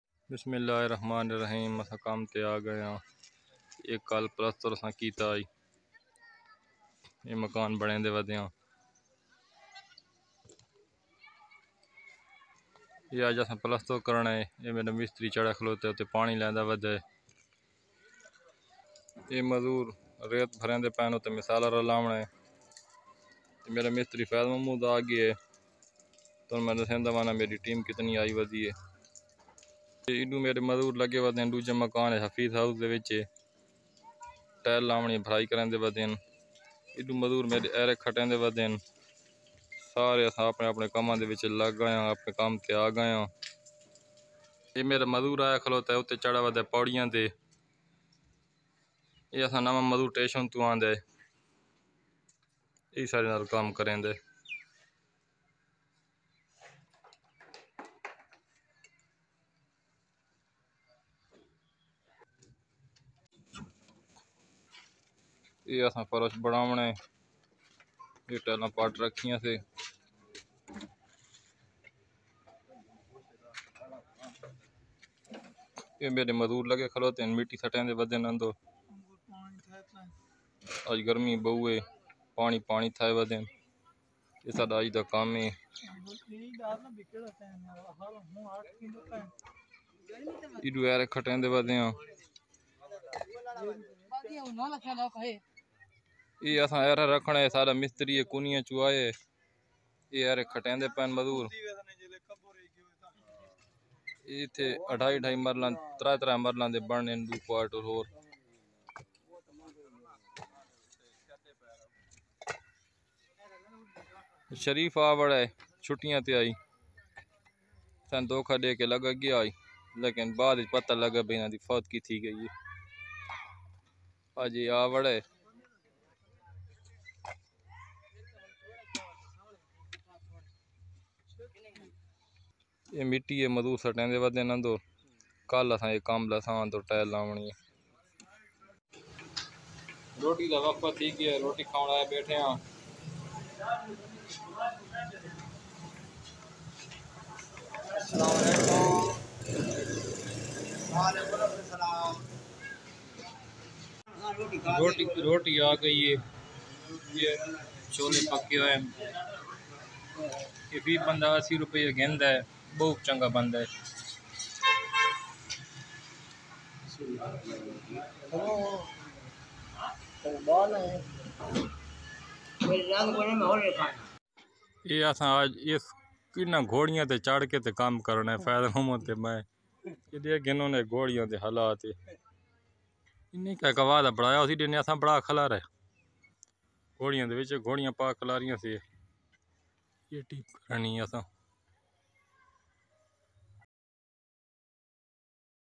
Aj k din kam ki routine|daily update|Maan wattakhel.
AJ ki video me Rozana k construction k Kam ki highlights hen
Pura din kia kia Kam hua
team members ka introduction
hard work
Life of labour
life of a poor man